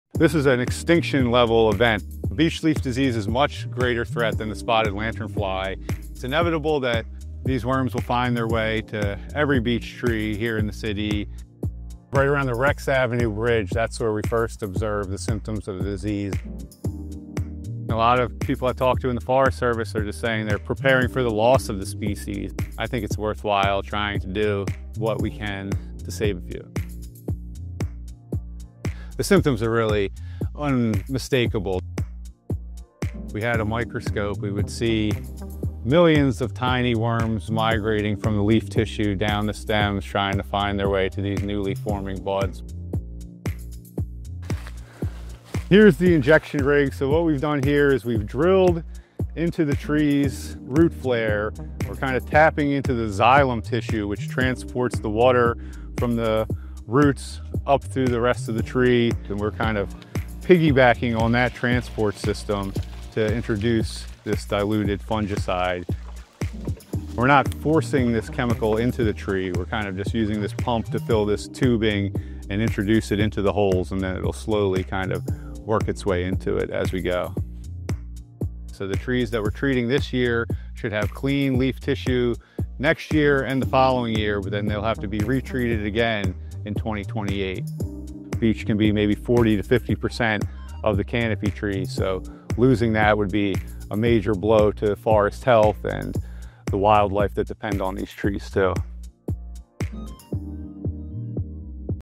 A new disease is killing Philly's beech trees. This treatment could save some of them
Philly’s beech trees are under siege. Thousands are succumbing to the relatively new and dreaded beech leaf disease caused by a tiny worm, or nematode.

Dhan Parker, an environmental scientist with Philadelphia Parks and Recreation, is working to save as many beech as he can — starting in the Wissahickon.

While there is no agreed upon treatment yet for beech leaf disease, foresters and scientists like Parker are using fungicides such as thiabendazole and fluopyram to slow the spread. It can take up to three hours to treat a single tree.

Beeches aren’t the only endangered tree in our region’s forests. Ash is under attack by the emerald ash borer, eastern hemlocks by the wooly adelgid, and oaks face multiple threats. (Parker is currently treating 1,500 ash trees in the city on a rotating basis.) Woody invasive vines are also strangling native vegetation and preventing natural regeneration. All this combined can lead to changes in a forest’s overstory and understory, inevitably altering the composition of the woods.

More about the diseases and pests attacking our trees, as well their deadly consequences